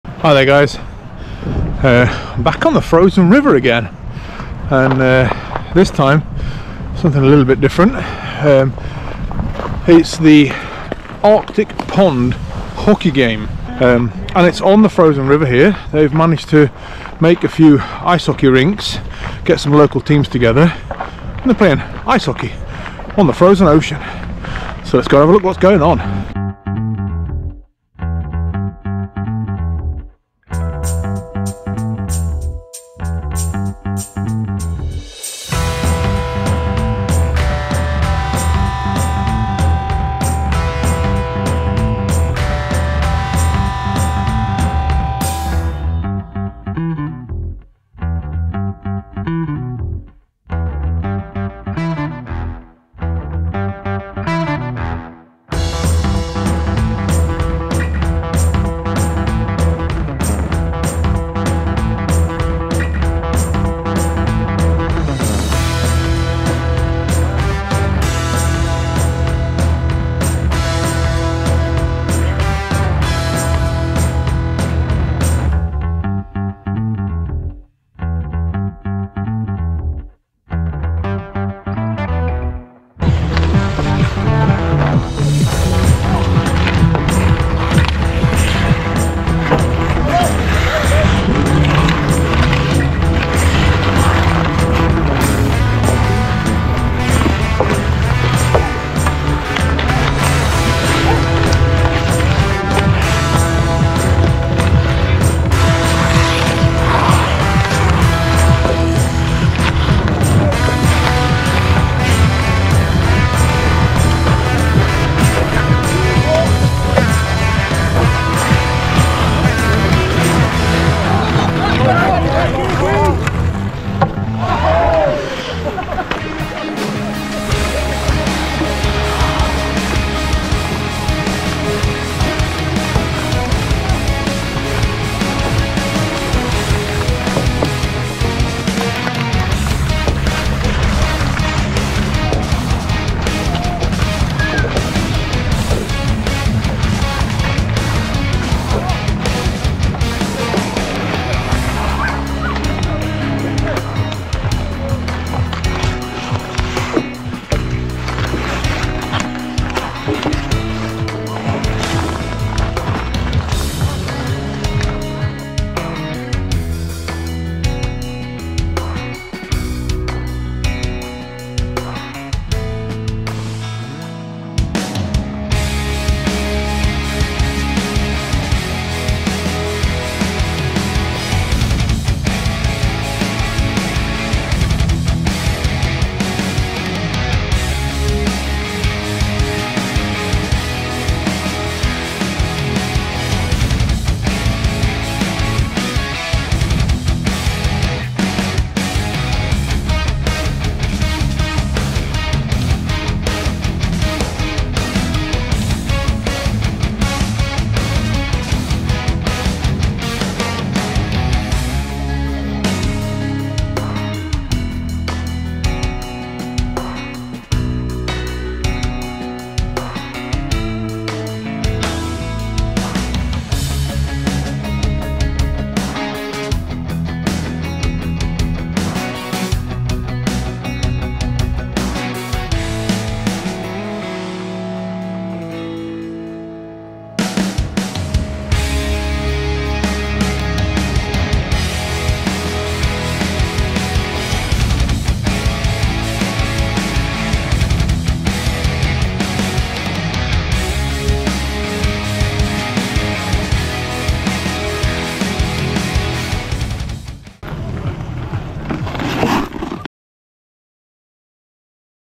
Playing ice hockey on the frozen Baltic sea. Artic pond hockey games 2023.
Saturday March 4 2023 is the premiere for Sweden's northernmost outdoor ice hockey tournament.

In this video, we take you on an exciting journey to the frozen Baltic Sea where we witness the exhilarating game of ice hockey. The players glide across the frozen terrain with ease, their skates digging deep into the ice as they chase after the puck. You'll witness breathtaking shots, intense battles on the rink. From the fast and furious pace of the game to the breathtaking scenery that surrounds them, this video has it all. You'll see the players give it their all as they attempt to outmaneuver their opponents and take home the gold. Whether you're a die-hard hockey fan or just love watching people push themselves to the limit, this video is definitely one you won't want to miss. So sit back, relax, and get ready for some hockey on the Frozen Baltic Sea.

With the oceans natural ice under the skate rails and Luleå's city skyline in the background, it can't be anything other than magical!

Arctic Pond Hockey Games 2023 offers nerve-wrecking matches, good hockey fun and amazing scenery.
Visit the north of Sweden and you too can experience the extremes northern sweden has to offer. From midnight sun to -30 and the frozen Baltic sea.

LuleaLulea winterLuleå swedenMike the mechanic